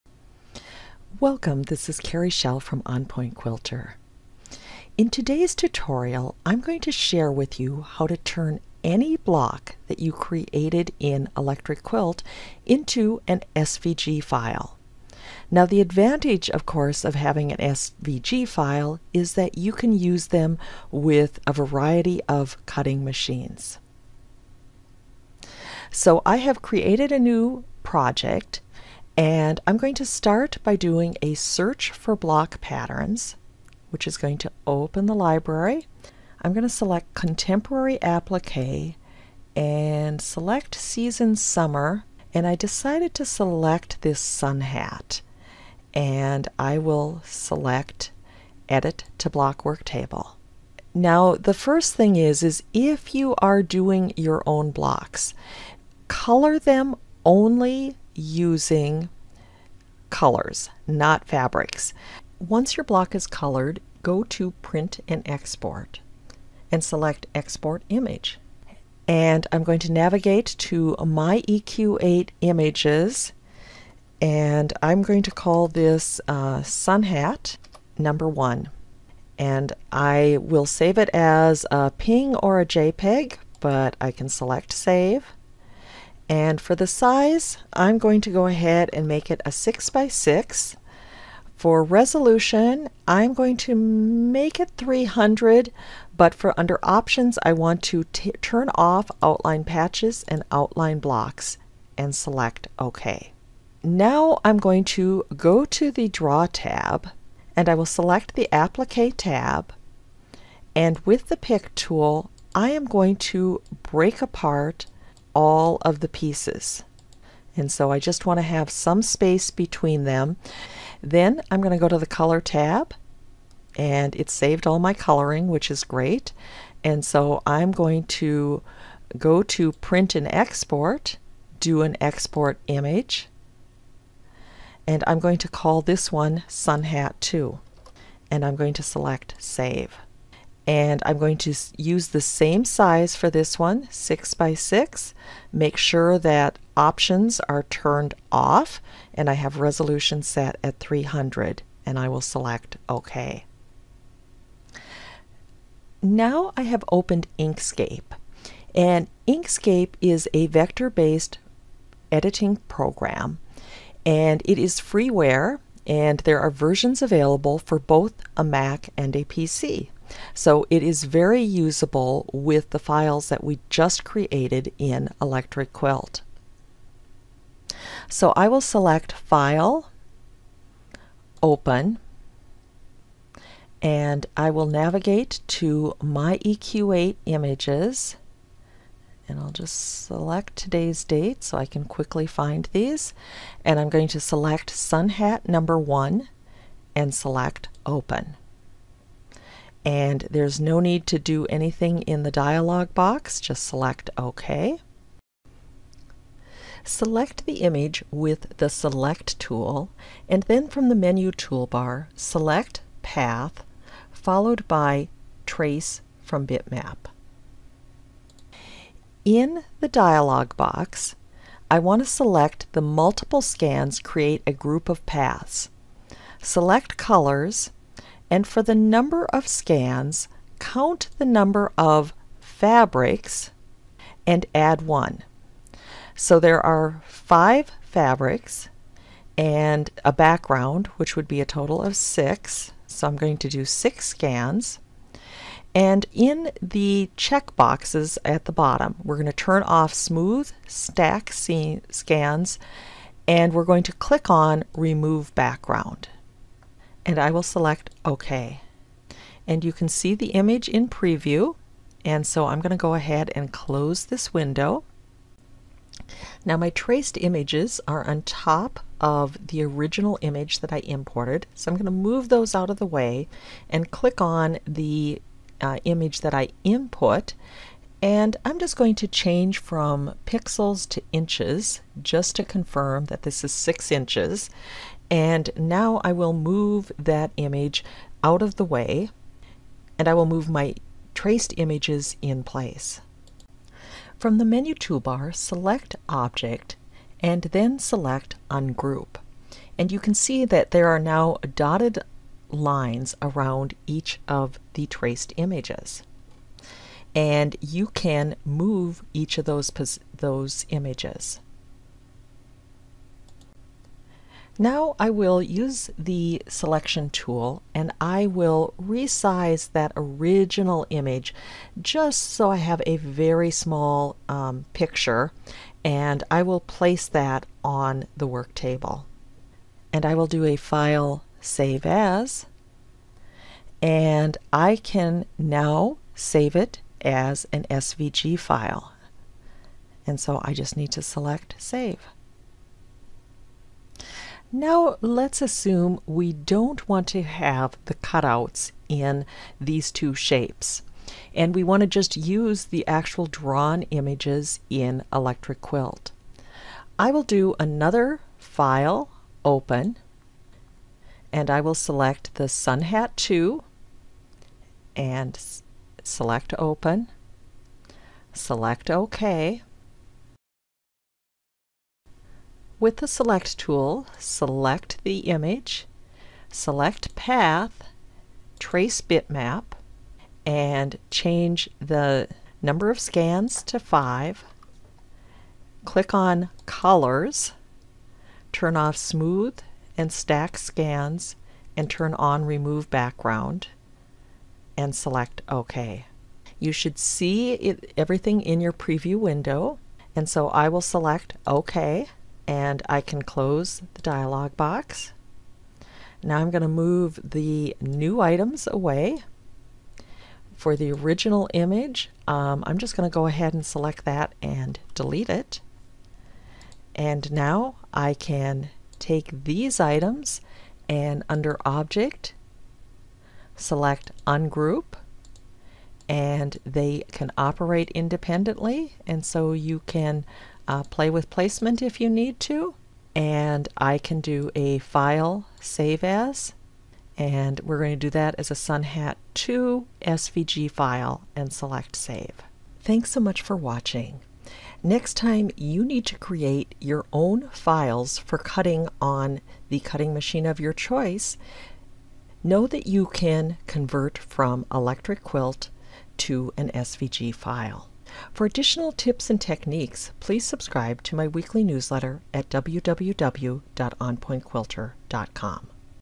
Creating SVG Files from Electric Quilt Blocks and Quilts - An Electric Quilt 8 Tutorial
In a Facebook group this week, there was a discussion on creating files that could be used by cutting machines such as Silhouette Cameo, Brother Scan and Cut and Cricut Explore Air and Cricut Maker.  These cutting systems all have proprietary software that makes sharing designs between systems problematic.  However --- all of them allow for use of SVG files.

Since Electric Quilt only exports to raster formats, we need to be able to transition from those formats to SVG.

In my free Electric Quilt video tutorial, I will share how to go from a block in Electric Quilt to an svg file.  The intermediate software I use is Inkscape. Inkscape is a professional vector graphics editor for Windows, Mac OS X and Linux. It's free and open source.  It can seem overwhelming to use when you open it for the first time. However, I will share with you the few items you need to create your SVG file from Electric Quilt.

Once you have it downloaded, it is super simple to convert your files.  For additional information, check out the related blog post